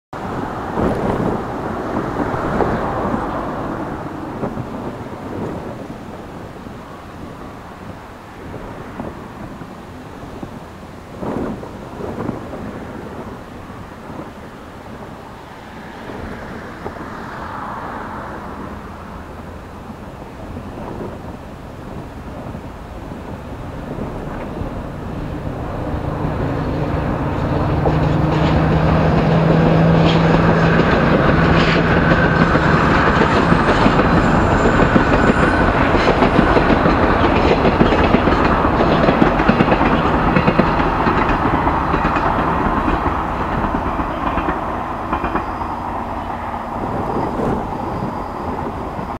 JR (団臨）旅の贈りもの号 ３日目（ワンカット）
映画『旅の贈りもの』のプロモーションで運転された募集団臨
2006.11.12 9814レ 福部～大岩